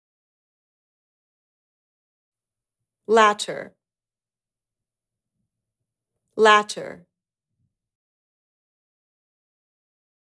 How to pronounce LATTER in American English
This video shows you how to pronounce LATTER in American English. Speaker has an accent from New York City, NY. Collins Dictionary, the home of living English and pioneers of dictionary publishing